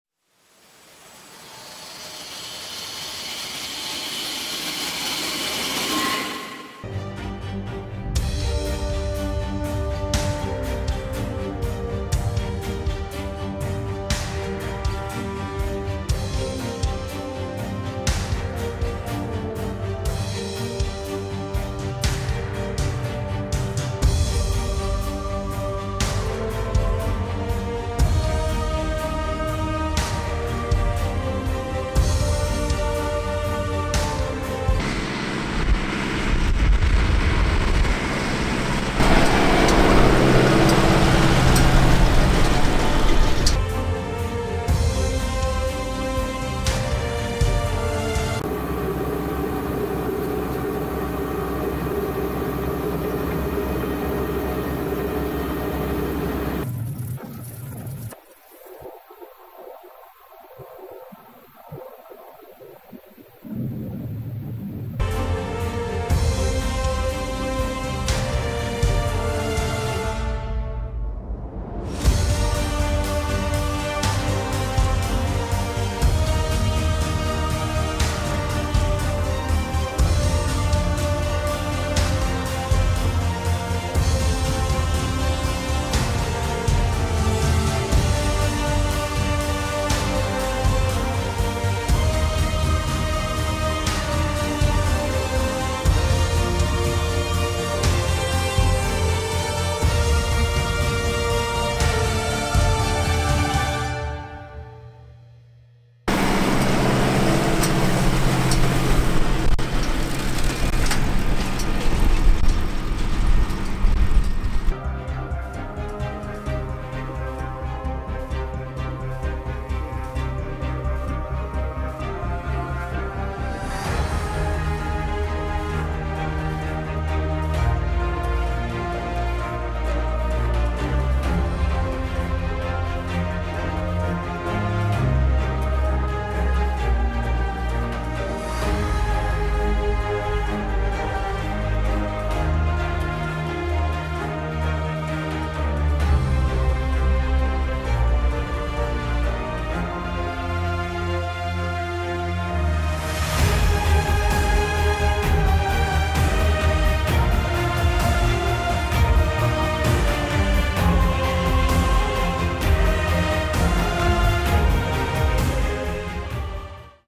Borst Haybaling & Fourage l balenpersen en wikkelen l Fendt 309 l Criap Midi-Baler 85
Borst Haybaling & Fourage aan het balenpersen met een Fendt 309 en een Criap Midi-baler 85. De balen worden meteen gewikkeld en neergelegd op het veld. Wilt u ook balen geperst hebben en gewikkeld?